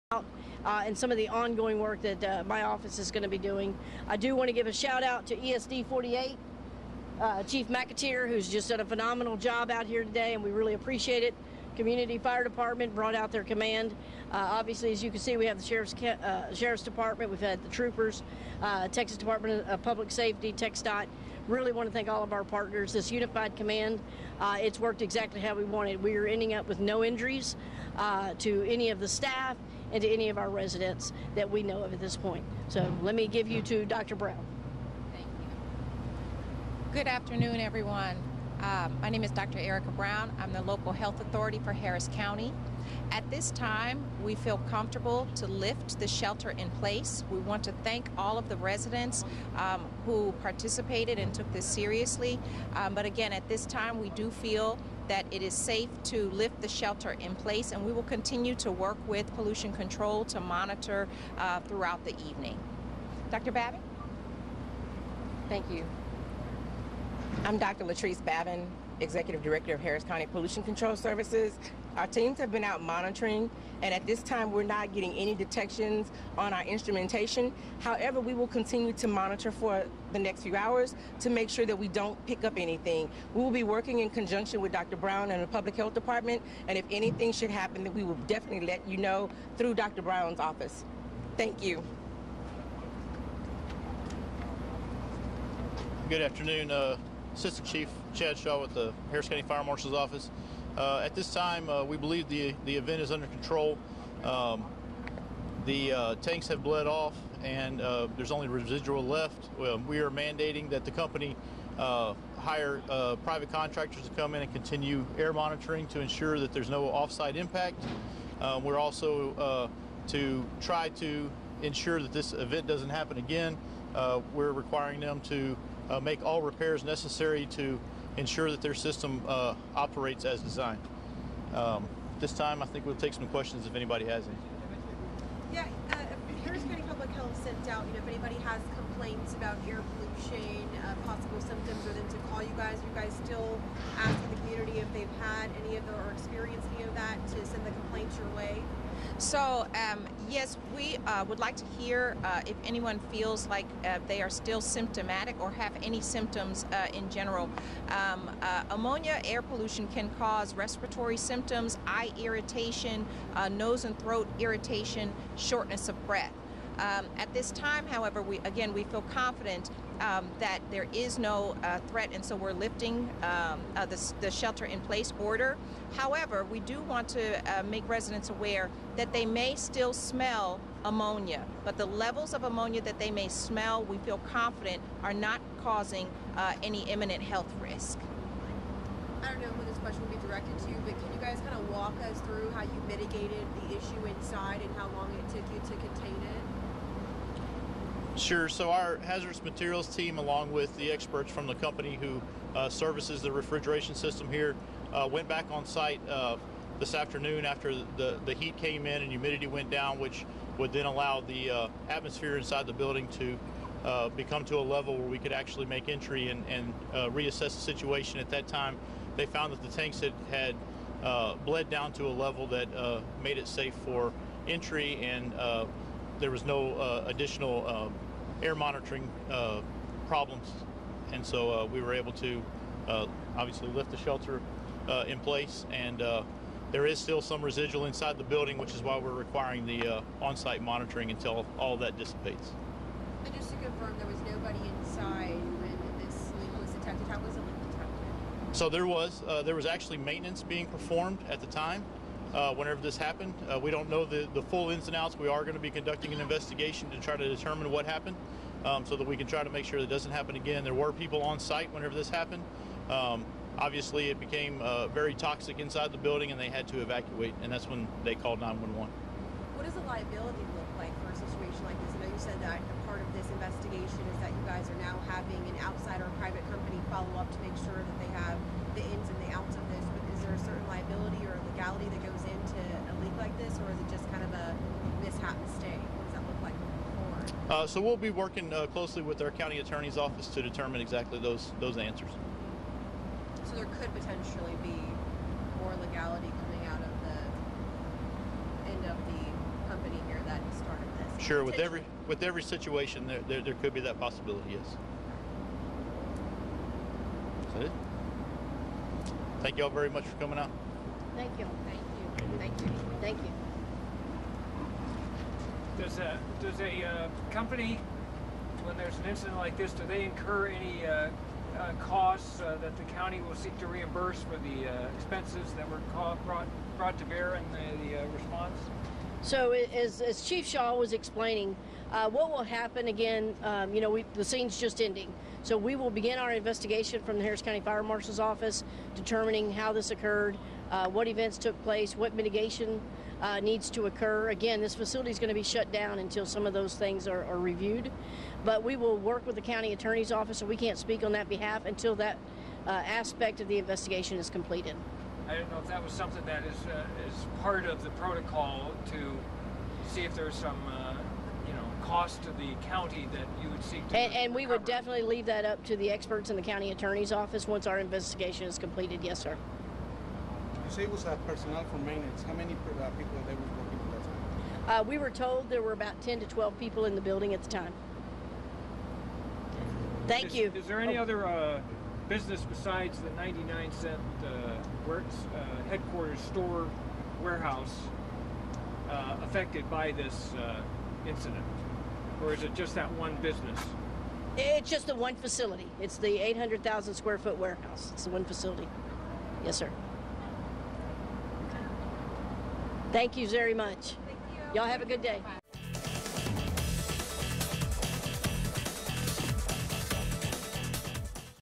Shelter in place lifted after chemical leak contained, officials say
Authorities said anhydrous ammonia began leaking from a 99-Cent Only store warehouse in Katy Saturday night and continued midday Sunday.